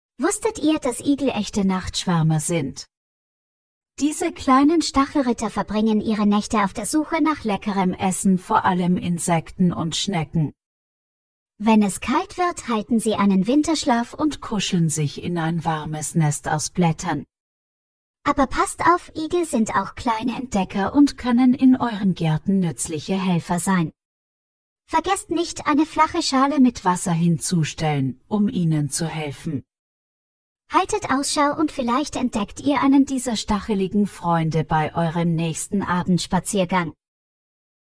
Der Igel 🦔 - Nachtschwärmer 🥳 und Nutztier
Igel sind Nachtschwärmer - Diese kleinen Stachelritter verbringen ihre Nächte auf der Suche nach leckerem Essen – vor allem Insekten und Schnecken! Wenn es kalt wird, halten sie einen Winterschlaf und kuscheln sich in ein warmes Nest aus Blättern. Aber passt auf: Igel sind auch kleine Entdecker und können in euren Gärten nützliche Helfer sein. Vergesst nicht, eine flache Schale mit Wasser hinzustellen, um ihnen zu helfen. Haltet Ausschau und vielleicht entdeckt ihr einen dieser stacheligen Freunde bei eurem nächsten Abendspaziergang!